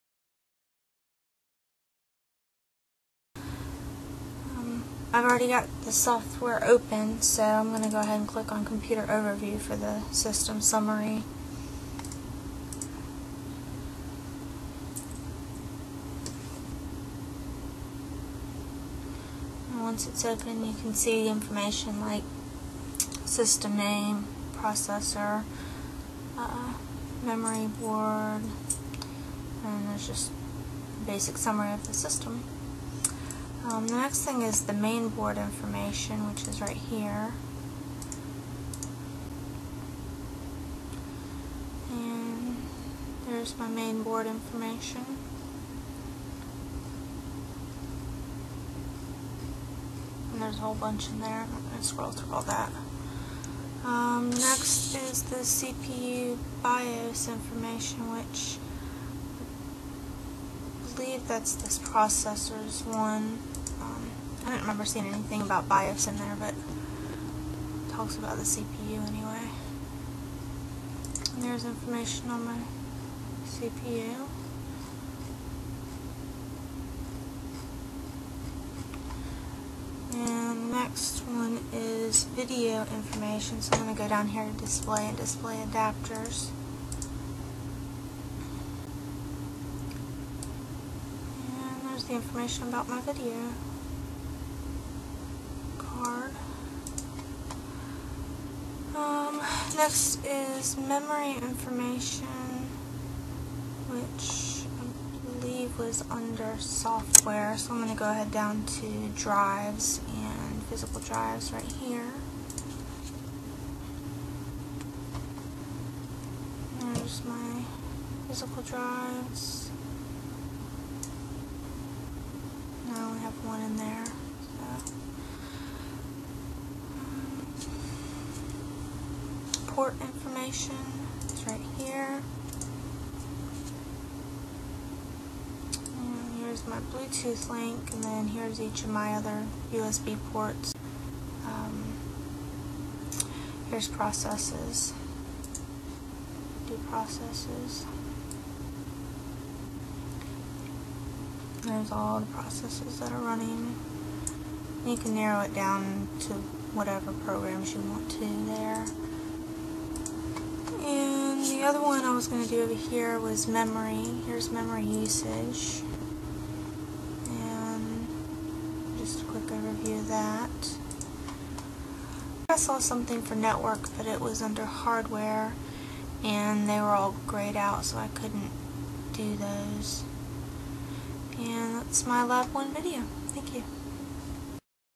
SiSoft Sandra
Lab done for my class at WKU. It is on SiSoft Sandra a program to collect information about your computer system.
Sorry about the quality. I was still experimenting with the recording program.